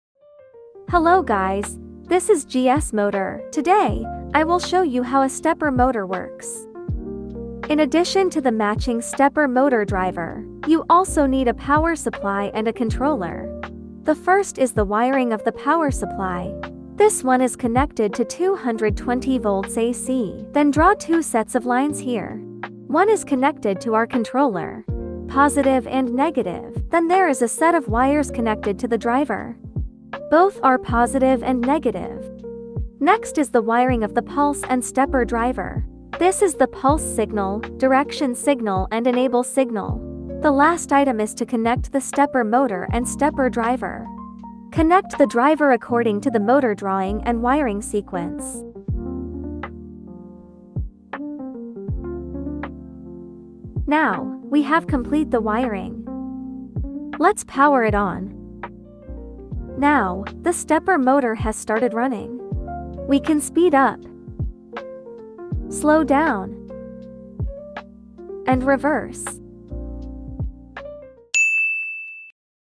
One Minute Stepper Motor Wiring Guide
Struggling with stepper motor wiring? We've got you covered! In this quick guide, learn how to connect the Stepper Motor, Driver, Power Supply, and Controller for smooth operation.

 Key Steps:
Connect power supply to controller & driver
Wire driver to pulse, direction, and enable signals
Attach motor to driver in A+/A-, B+/B- order
Power on & control motion with ease!

Watch now and get your motor running in no time!